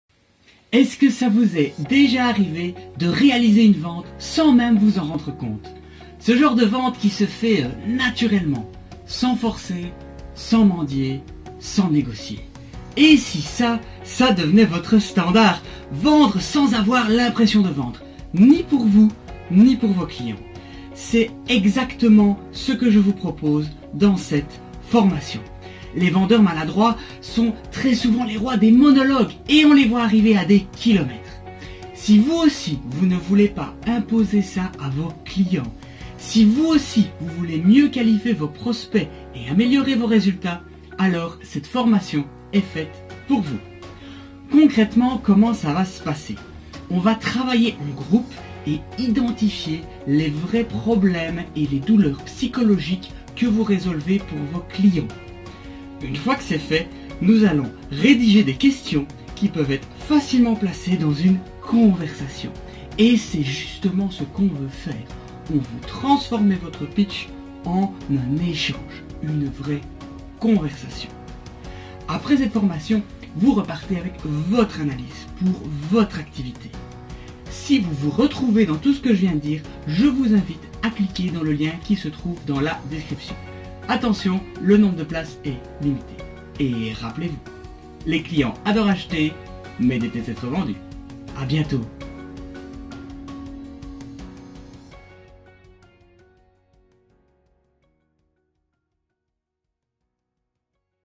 Présentation de la formation « Arrêtez de vendre, ayez des conversations »
Est-ce que ça vous est déjà arrivé de réaliser une vente sans même vous en rendre compte, sans forcer, sans négocier ?

Et si ça, ça devenait votre standard ? Quel impact cela aurait sur vous et sur vos affaires ?

Vendre sans avoir l’impression de vendre, ni pour vous, ni pour vos clients !

C’est exactement ce que Nicolas Portetelle vous propose dans cette formation qui aura lieu au Coworking Fernelmont le 15 mars 2019 de 9 h à 12 h.